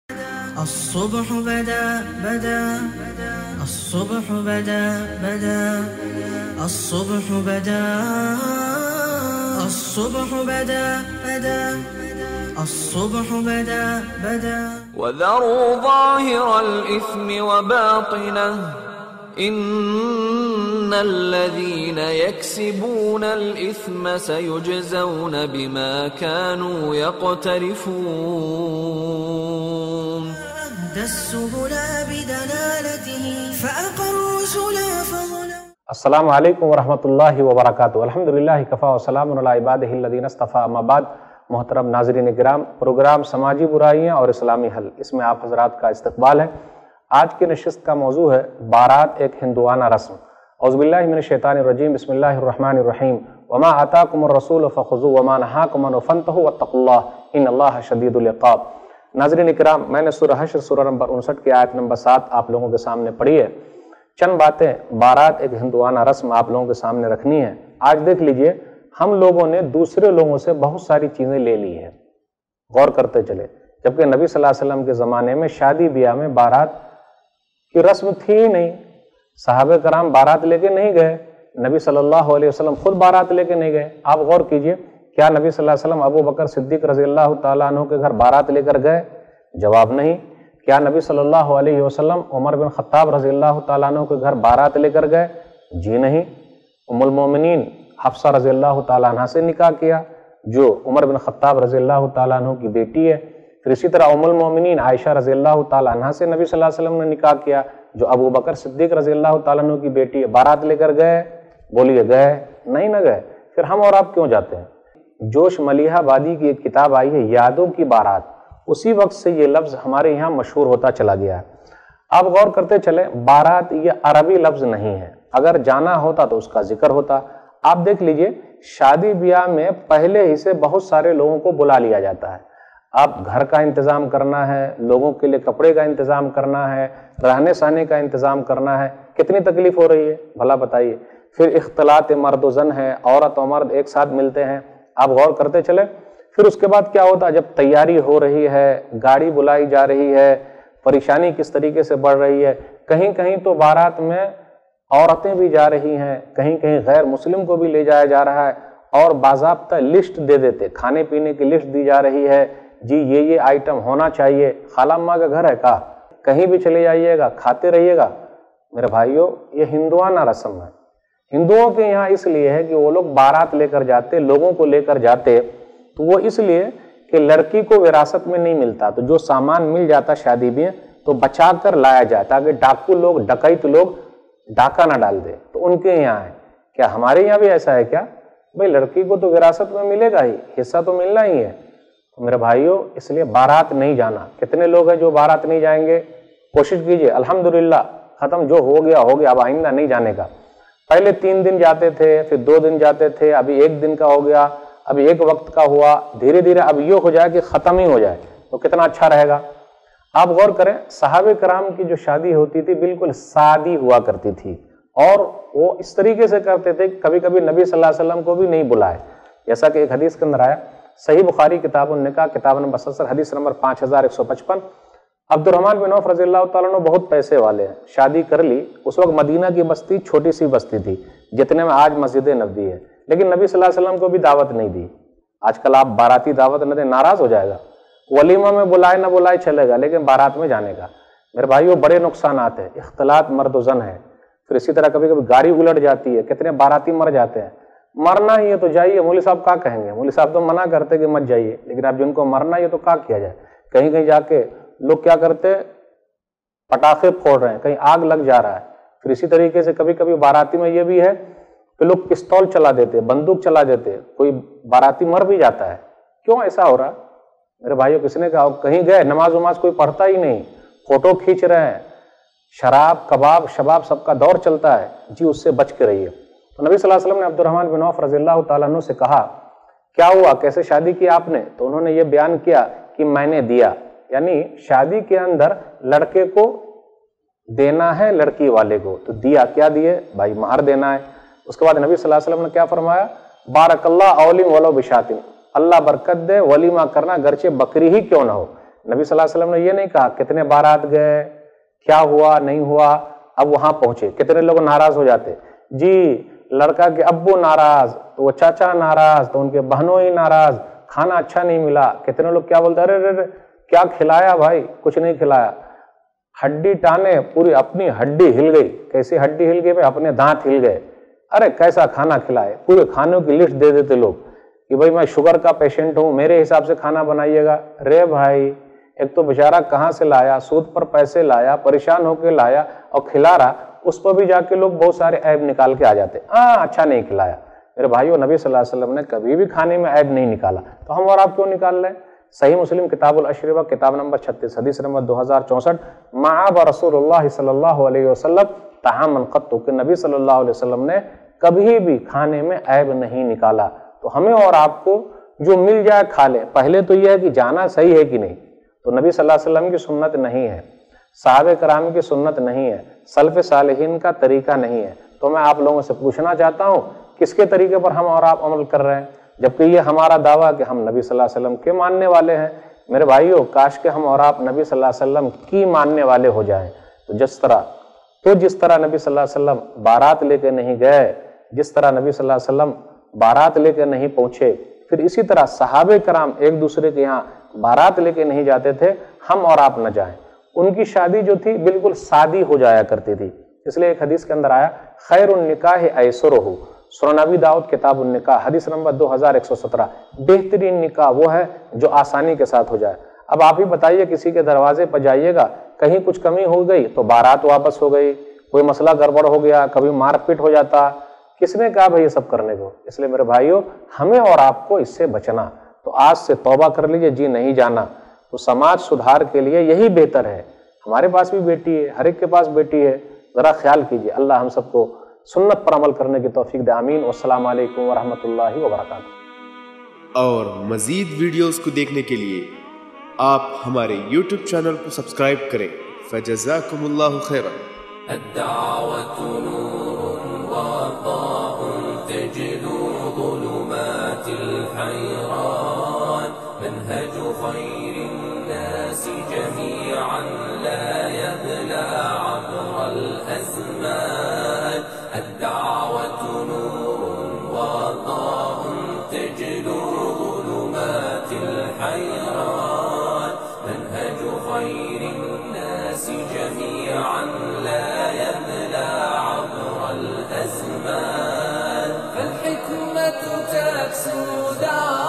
13) Baraat - ek Hindu Riwaaj || Shaikh Sanaullah Madani || Darul Huda
Topic : Baraat - ek Hindu Riwaaj
Series : Samaji Buraiyan aur Islami Hal
Speaker : Shaikh Sanaullah Madani, Darul Huda Speaker
Post Productions : Alauddeen Productions